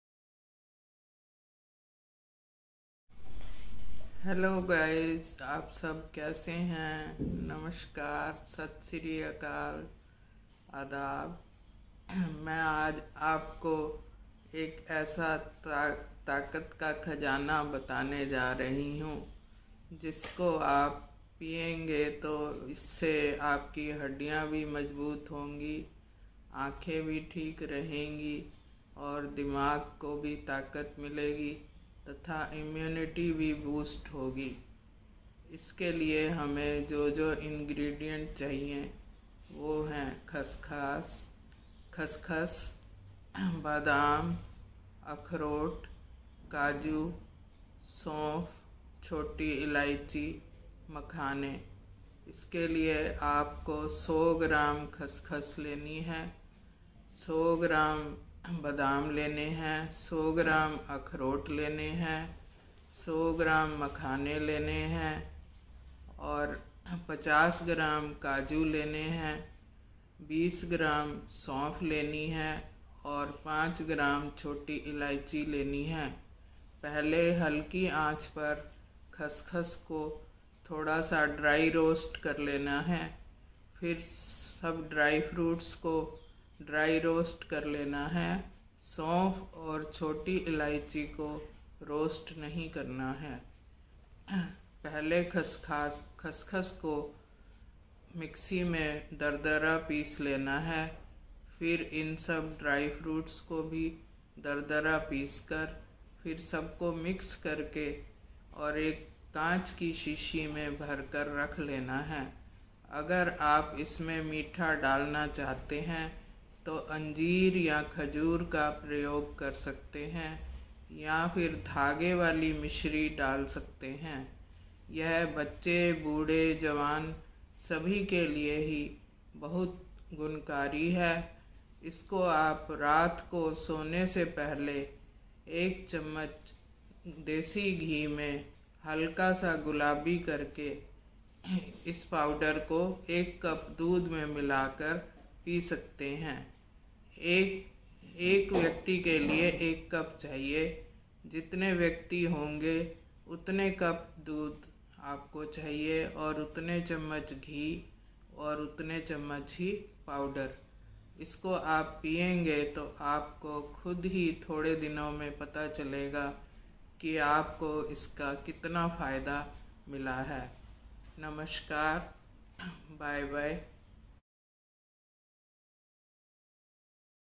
Taakat Ka Khazana
This video is a natural home remedy for improved sleep quality, weight gain, and strength, immunity and good for overall health for senior citizens.